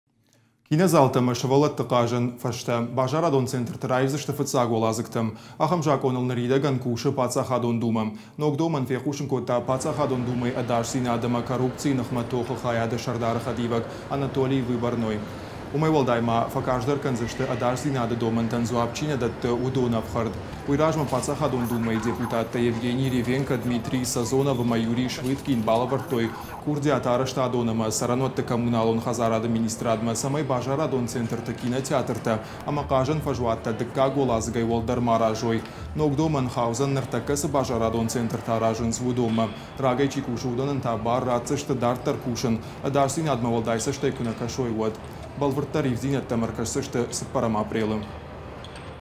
Кинозалтæ æмæ сывæллæты хъазæн агъуыстытæ базарадон центрты раивдзысты фыццаг уæладзыгтæм
Ахæм закъоныл ныридæгæн кусы Паддзахадон думæ. Ног домæн фехъусын кодта Паддзахадон думæйы æдасдзинады æмæ корупцийы ныхмæ тохы хайады сæрдары хæдивæг Анатолий Выборный. Уымæй уæлдай ма, фæкарздæр кæндзысты æдасдзинады домæнтæн дзуапп чи нæ дæтты уыдоны æфхæрд. Уый размæ Паддзахадон думæйы дупетаттæ Евгений Ревенко, Дмитрий Сазонов æмæ Юрий Швыткин балæвæрдтой курдиат арæзтадон æмæ цæрæнуæтты комуналон хæдзарады министрадмæ цæмæй базарадон центрты кинотеатртæ æмæ хъазæн фæзуаттæ дыккаг уæлыдзыгæй уæлдæр ма аразой. Ног домæн хаудзæн ныртæккæ цы базарадон центртæ аразынц, уыдонмæ. Рагæй чи кусы, уыдонæн та бар ратдзысты дарддæр кусын, æдасдзынадмæ уæлдай цæстæй куы нæ кæсой, уæд. Бæлвырддæр ивддзинæдтæм æркæсдзысты 4 апрелы.